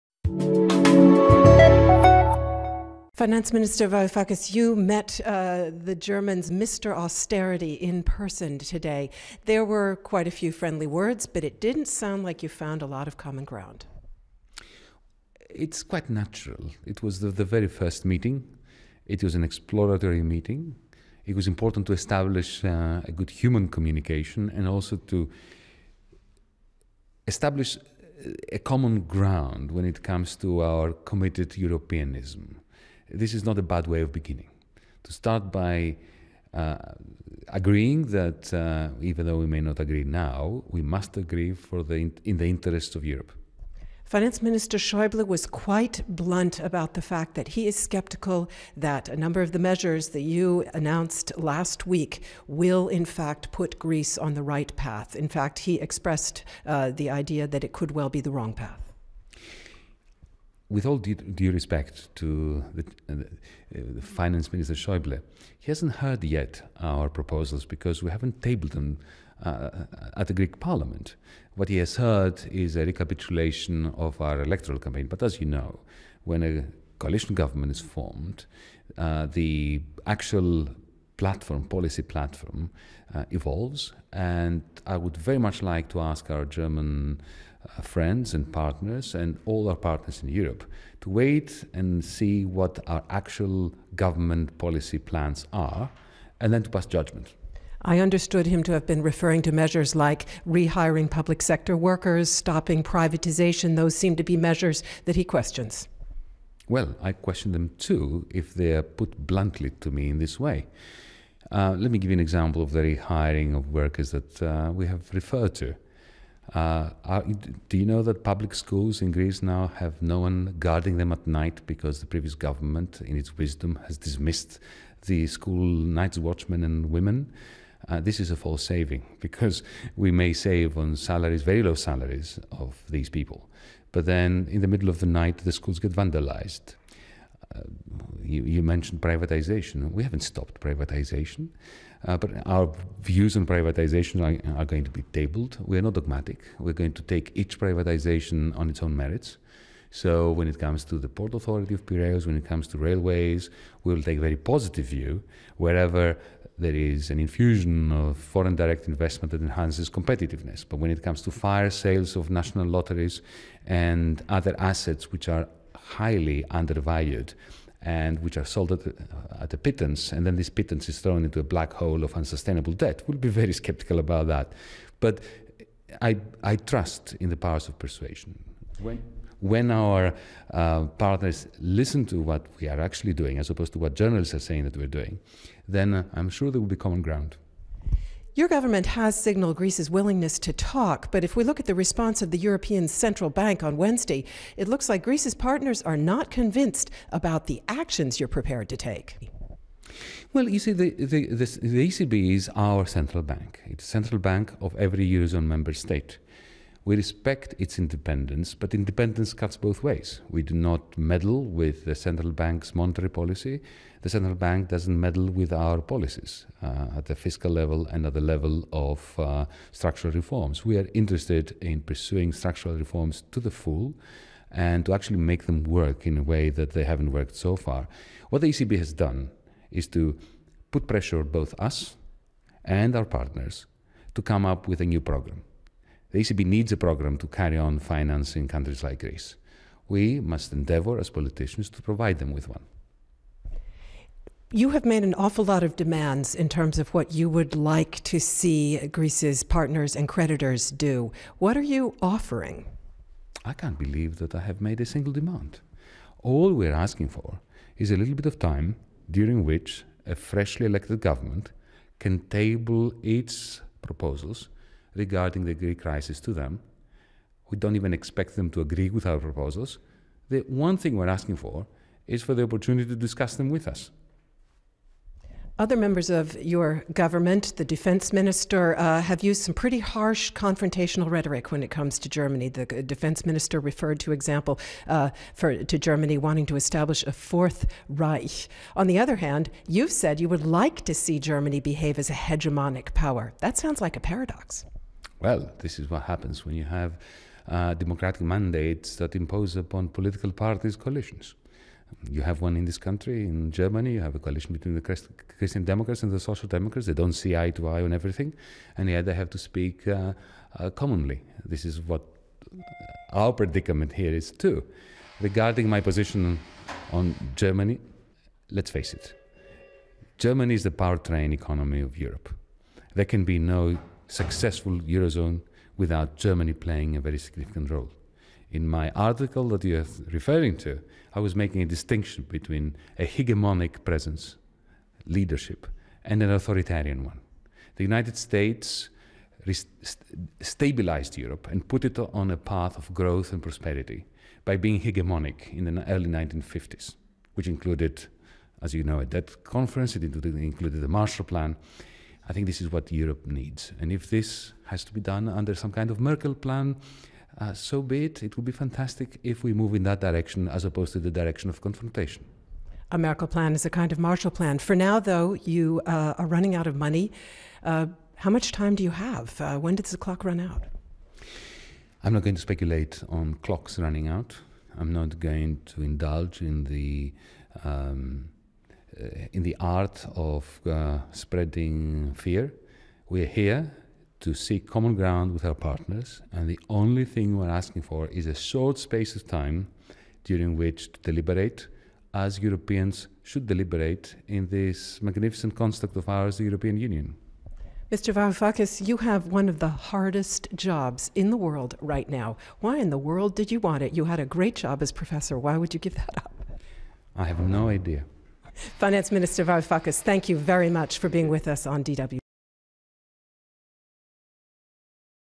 Greek Finance Minister in DW-Interview
DW's chief political correspondent Melinda Crane spoke to Mr. Varoufakis in an exclusive interview.